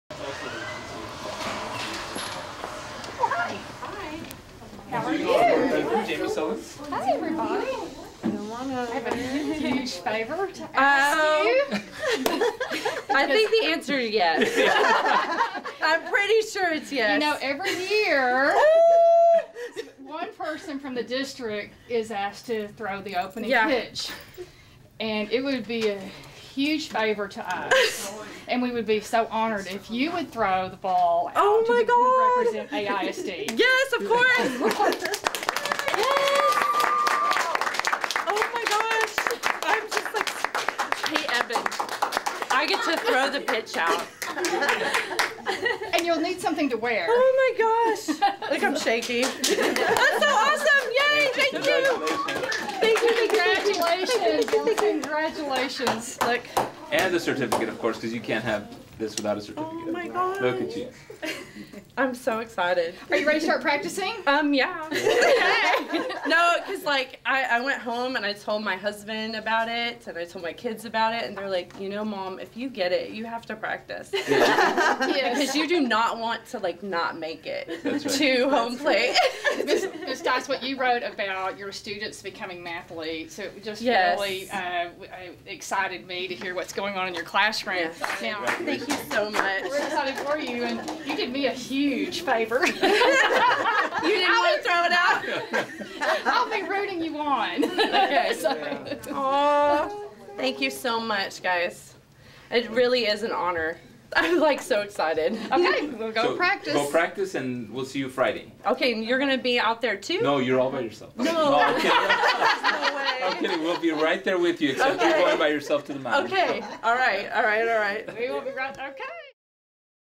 Rangers First Pitch Surprise
Pearcy Elementary teacher Jennifer Stice was surprised by the announcement that she won the honor of throwing out the first pitch at AISD Night at the Texas Rangers on Friday, May 13, 2016.